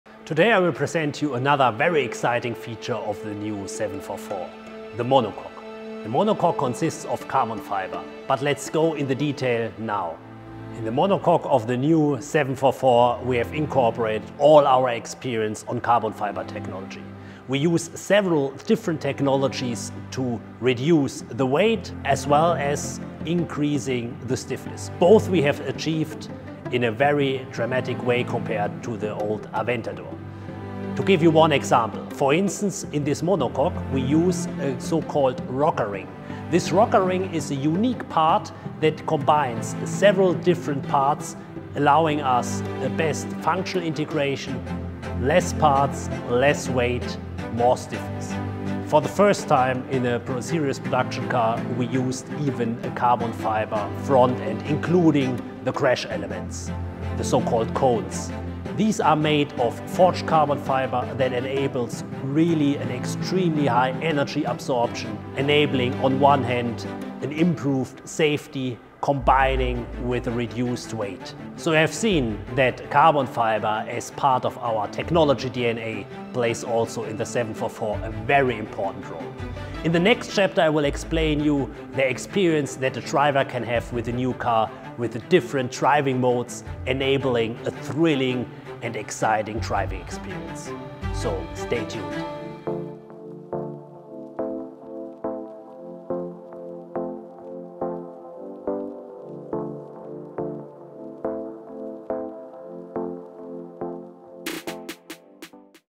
Lamborghini Revuelto Monofuselage REVEAL (Aventador Successor) | 100% Forged Composites
Just a few weeks ahead of launching the first hybrid plug-in V12 High Performance Electrified Vehicle (HPEV), the Sant’Agata Bolognese brand unveils a technical solution that is unique in today’s automotive landscape. The LB744 is based on a new aeronautics-inspired chassis, the ‘monofuselage’. As well as a monocoque made entirely of multi-technology carbon fiber, it features a front structure in Forged Composites; a special material made of short carbon fibers soaked in resin.

Source : Lamborghini